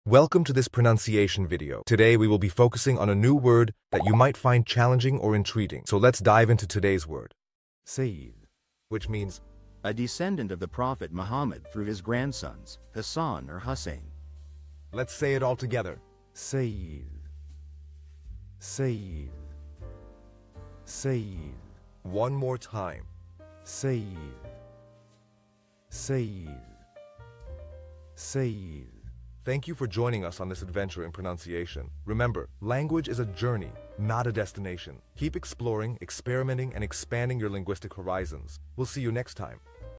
How to pronounce Sayid
Master the Pronunciation of 'Sayid WHICH MEANS SAYID' - which means : The Descendant of the Prophet Muhammad Through His Grandsons, Hasan or Husayn. 📝 with @PortuguesePronunciation 🎓🗣️ - [Your Guide to Portuguese]

Learn to pronounce 'Sayid' perfectly in Portuguese with our easy and engaging tutorial by @PortuguesePronunciation! 📖📽️ In this comprehensive video, we provide you with native speaker examples 🗣️👂 and phonetic breakdowns 🎵 to ensure you get the pronunciation just right.

How to say Sayid in Portuguese